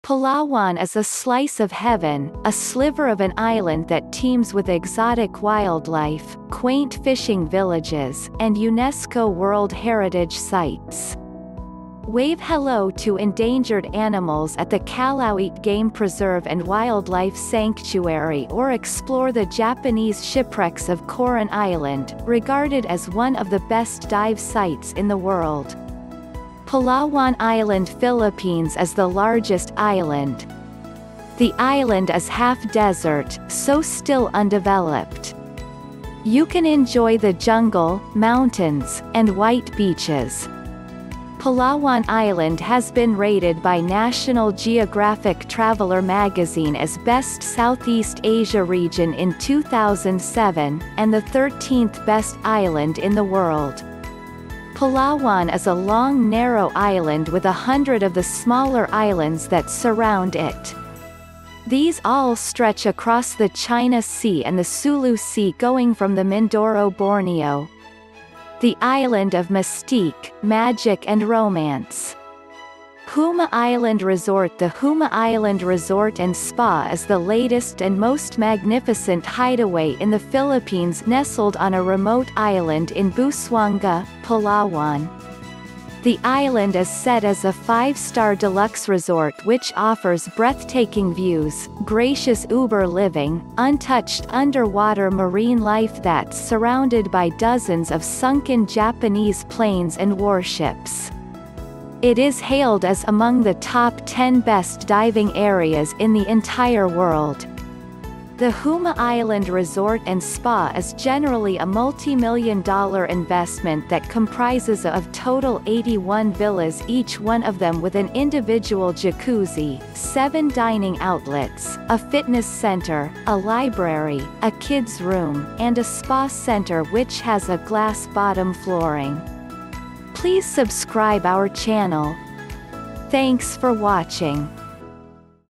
Palawan island tourism best of Palawan island
Palawan is a slice of heaven, a sliver of an island that teems with exotic wildlife, quaint fishing villages, and UNESCO World Heritage Sites. Wave hello to endangered animals at the Calauit Game Preserve and Wildlife Sanctuary or explore the Japanese shipwrecks of Coron Island, regarded as one of the best dive sites in the world.